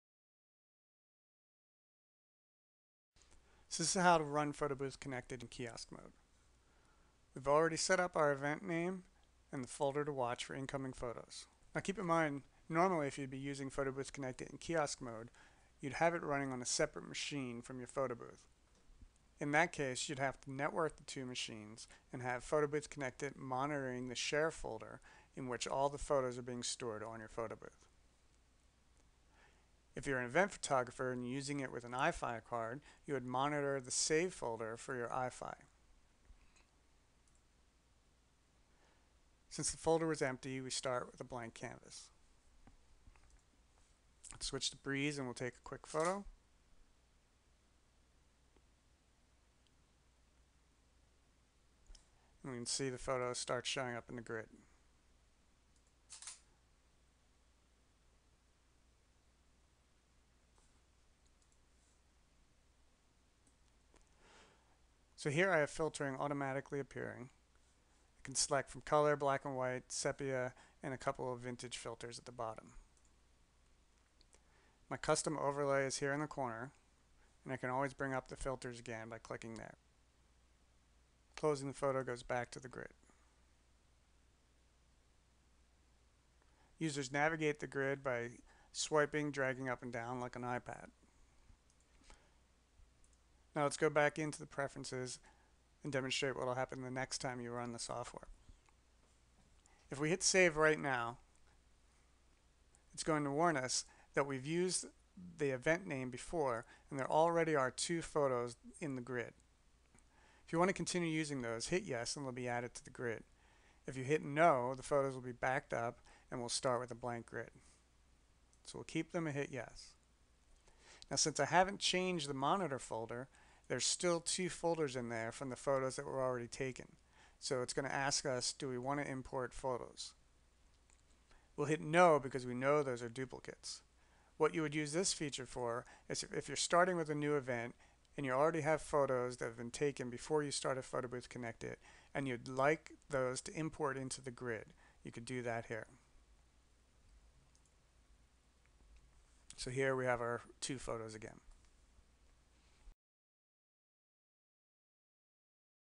Photo Booth Connected - Kiosk Setup
Set up a social media upload and print station at any event, or add more features right at your booth. Photo Booth Connected works with your existing photo booth software or Eye-Fi card to allow users to Filter, Email, Upload to Facebook, Send to Twitter, SMS and Print their photos.